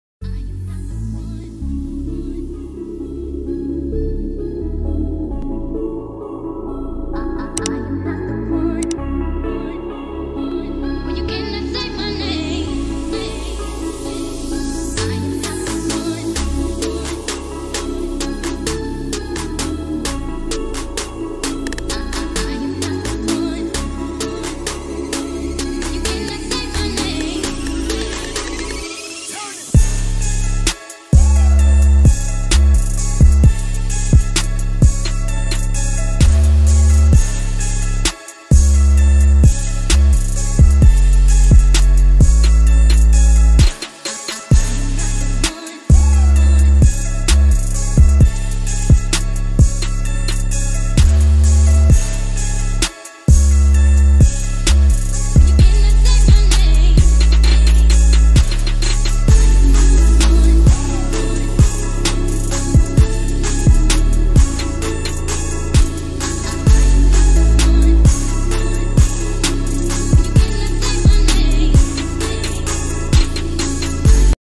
Manny Khoshbin Fitness Workout, he start off his weekdays with weights💪
Manny Khoshbin fitness working very hard to obtain a good health

More videos you love are added DAILY.

Video - Manny Khoshbin tiktok videos